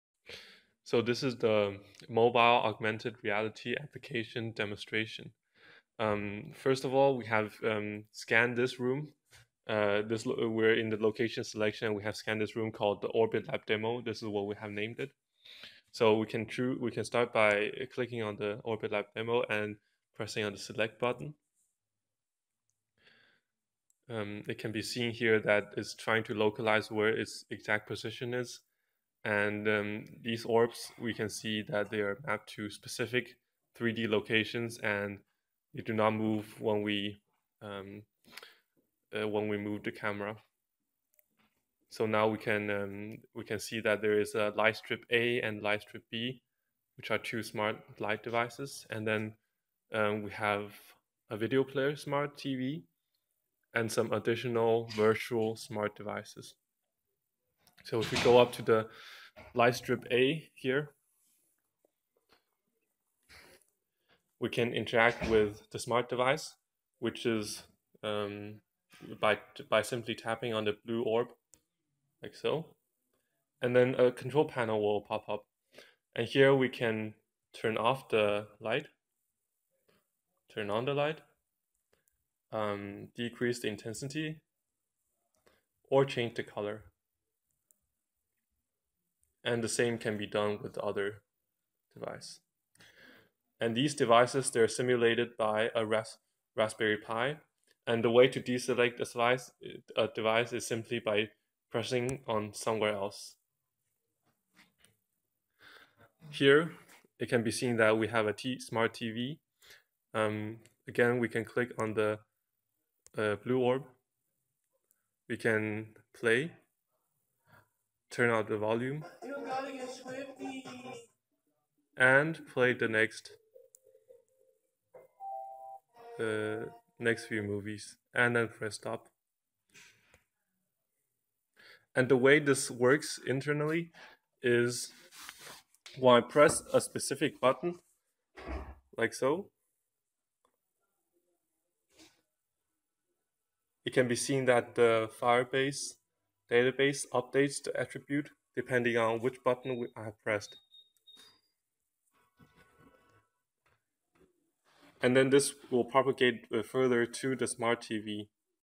Using Mobile Augmented Reality to Interact with IoT devices
This is the App developed by four engineering students - Naiyun Wu(me), Christian Legaard, Jacob Aagaard and Frederik Gadegaard. This is our bachelor'ss project.